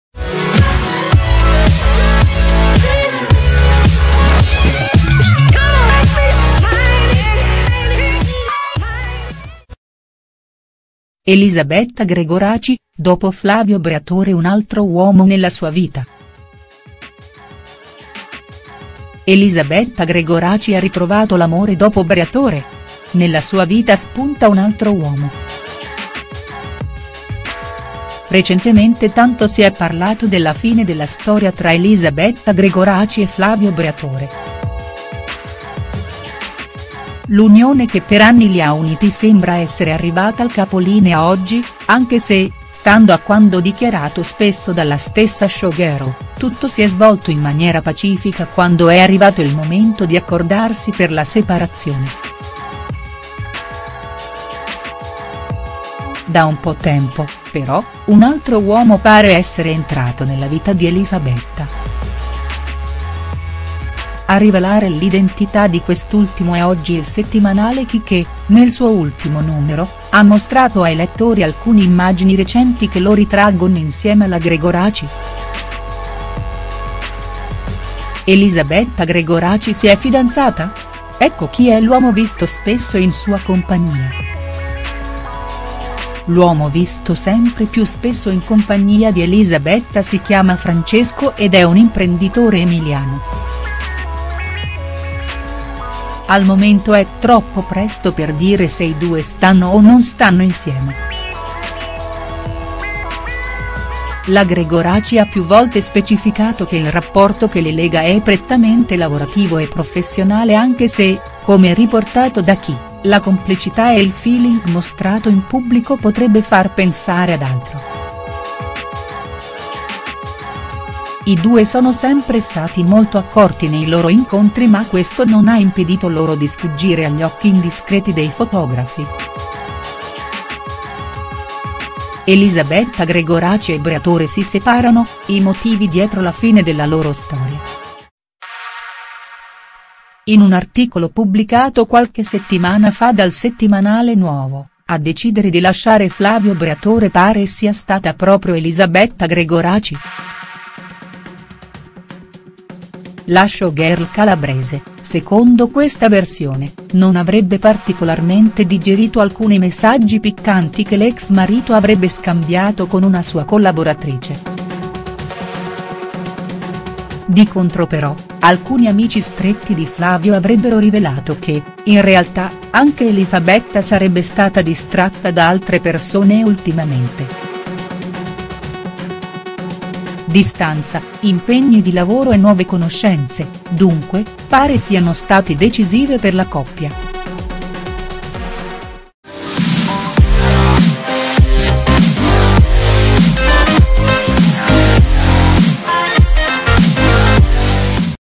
Elisabetta Gregoraci: dopo Flavio Briatore un altro uomo nella sua vita| STARS NEWS
Elisabetta Gregoraci ha ritrovato l'amore dopo Flavio Briatore? Nella vita della showgirl calabrese spunta un altro uomo.